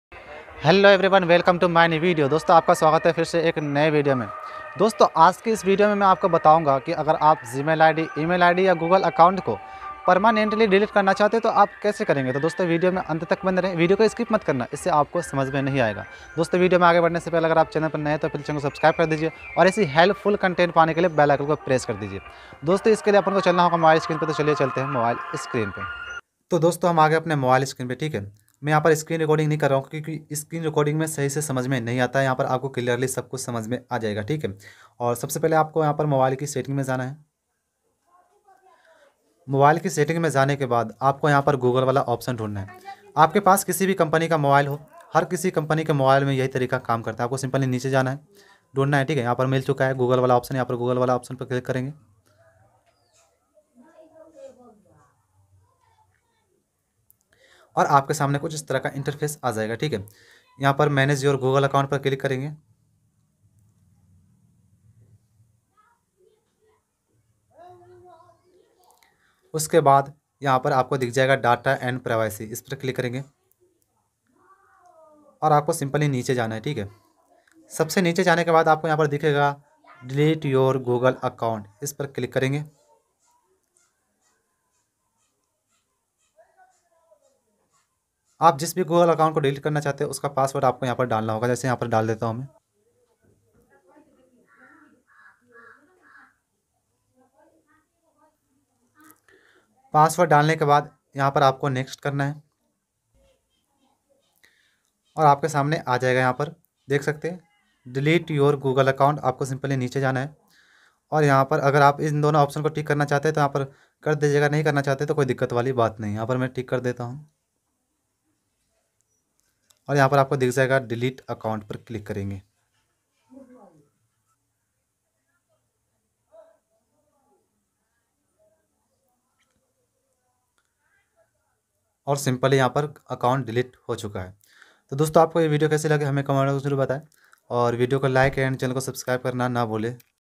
Email id kaise delete kare !! Gmail id delete kaise karen !! How to delete google account !!
Topic
Dosto aapka swagat hai fir se ak new video
mai aapko is video men btaunga ki aap gmail
id kaise delete karen,